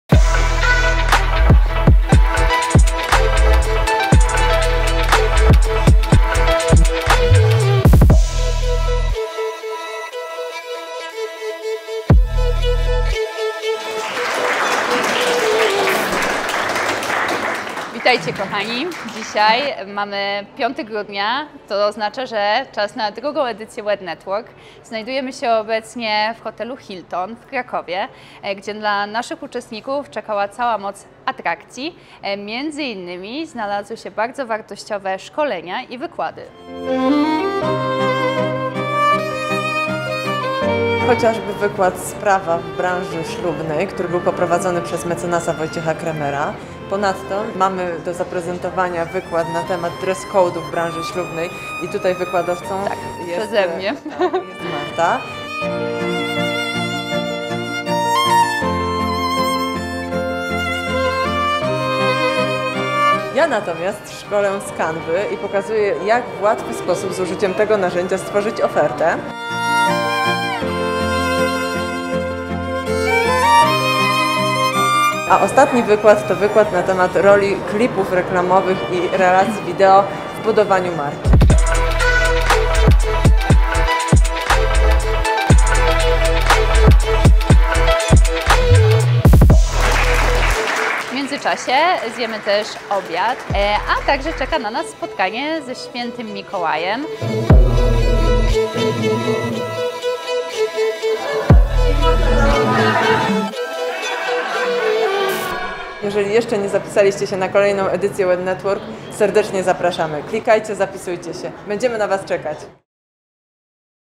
WEDNETWORK - szkoleniowo - integracyjne wydarzenie dla branży ślubnej. Film z drugiej edycji- Hilton
Zapraszam do obejrzenia relacji z drugiej edycji WEDnetwork, czyli zrzeszenia branży ślubnej! Tym razem zorganizowałyśmy szkolenia z zakresu: prawa dla branży ślubnej, marketingu w formie reklamy, canvy oraz zasad dress code. Dodatkowo integrowaliśmy się i wymienialiśmy się wizytówkami & doświadczeniami przy pysznym obiedzie i prosecco! Odwiedził nas również św. Mikołaj z prezentami :).

Kolejna edycja już w marcu, nie może Cię zabraknąć!!

Film by @malinowemedia